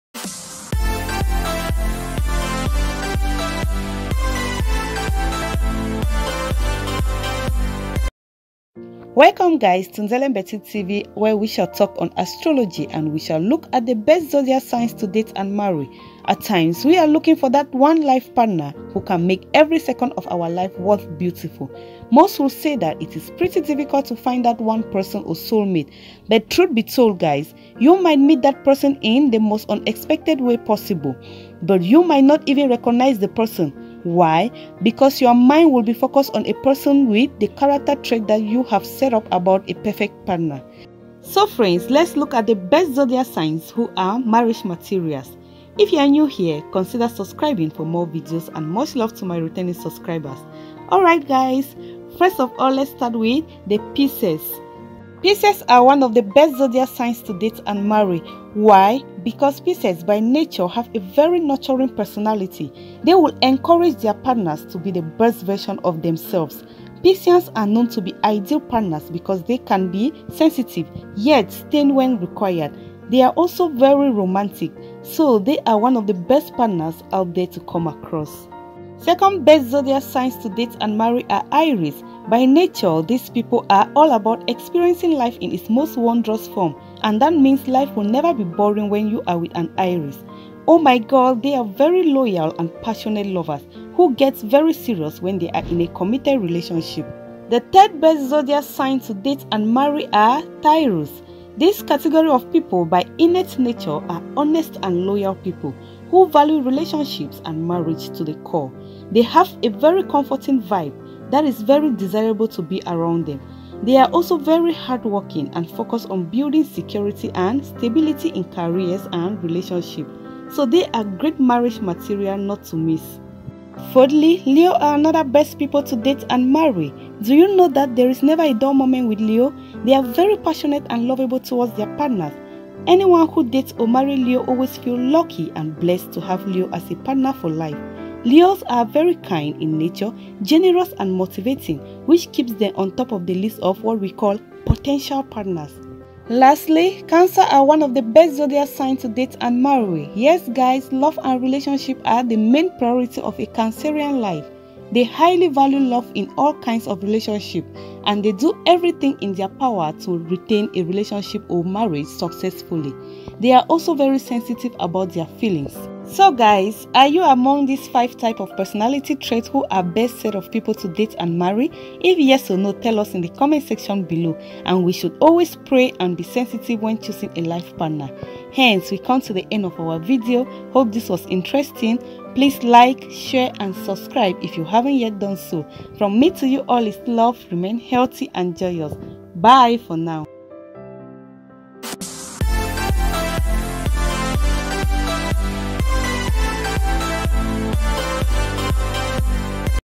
5#Best Zodiac Signs to date and marry without stress❤💃🏻⚘
hello friends in this vidoe we talk about the top best ZODIAC signs to date and get married without stress due to their innate born nature and personality traits. watch and you will enjoy it and see if you fall among .please if you are new subcribe and join us for more tips on this channel.